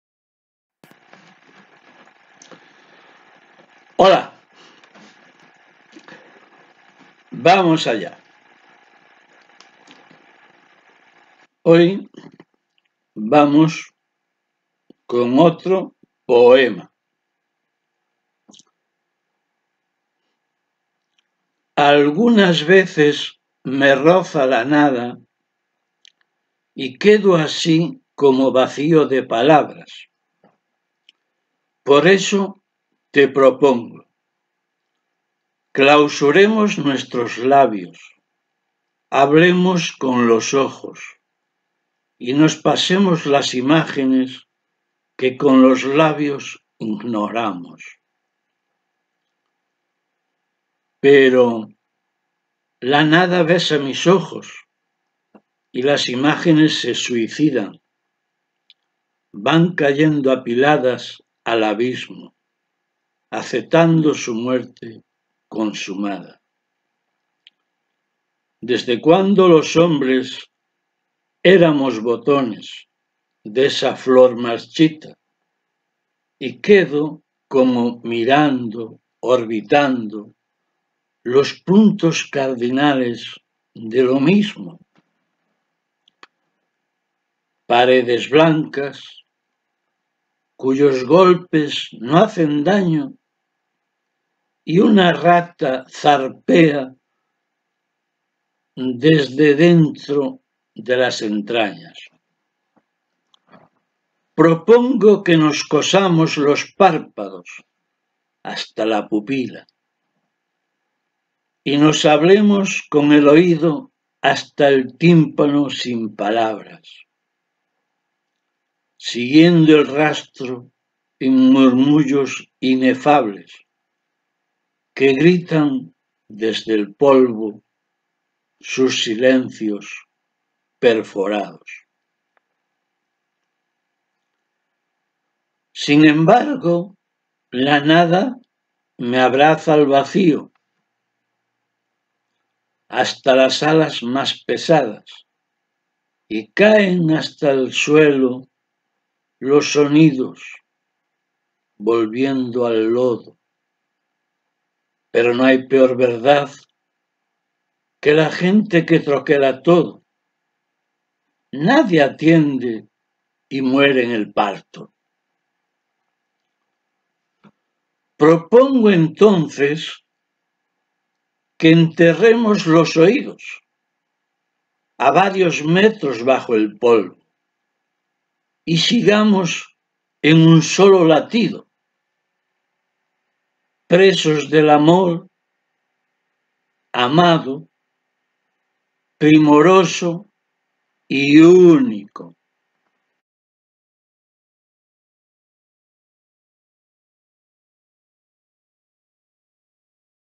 Te Propongo
Título: Te propongo

Algunas veces me roza la nada
y quedo así...
como vacío de palabras
Por eso, te propongo:
Clausuremos nuestros labios
hablemos con los ojos
y nos pasemos las imágenes
que con los labios ignoramos.

Pero
la nada besa mis ojos
y las imágenes se suicidan
van cayendo apiladas al abismo,
aceptando su muerte consumada
desde cuando los hombres
éramos botones... de esa flor marchita
y quedo como mirando, orbitando;
los puntos cardinales de lo mismo.

Paredes blancas
cuyos golpes no hacen daño y una rata
zarpea desde dentro las entrañas.
Propongo que nos cosamos
los párpados hasta la pupila,
y nos hablemos
con el oído hasta el tímpano
sin palabras, siguiendo el rastro
en murmullos inefables
que gritan desde el polvo
sus silencios perforados.

Sin embargo
la nada me abraza al vacío
hasta las alas más pesadas
y caen hasta el suelo los sonidos,
volviendo al lodo...
que no hay peor verdad
que la gente que troquela todo;
nadie atiende y muere en el parto.

Propongo, entonces
que enterremos los oídos
a varios metros bajo el polvo
y sigamos en un sólo latido,
presos del amor amado
primoroso y único.

Joaquín Lourido
Quino ©

I propose

Sometimes nothingness touches me
and it remains like this ...
like void of words
Therefore, I propose you:
Let's close our lips
let's talk with the eyes
and let's pass the images
that we ignore with our lips.

But
nothing kisses my eyes
and the images commit suicide
they are falling stacked into the abyss,
accepting his consummate death
since when men
we were buttons ... of that withered flower
and I am looking, orbiting;
the cardinal points of the same.

White walls
whose blows don't hurt and a rat
sail from within the entrails.
I propose that we sew
the eyelids to the pupil,
and let's talk
with the ear to the eardrum
speechless, following the trail
in ineffable murmurs
that scream from the dust
its perforated silences.

Nevertheless
nothingness embraces me in the void
even the heaviest wings
and the sounds fall to the ground,
going back to the mud ...
that there is no worse truth
that people who stamp everything;
nobody attends and dies in childbirth.

I propose, then
let us bury our ears
several meters under the dust
and let's continue in a single heartbeat,
prisoners of beloved love
exquisite and unique.

Joaquin Lourido
Quino ©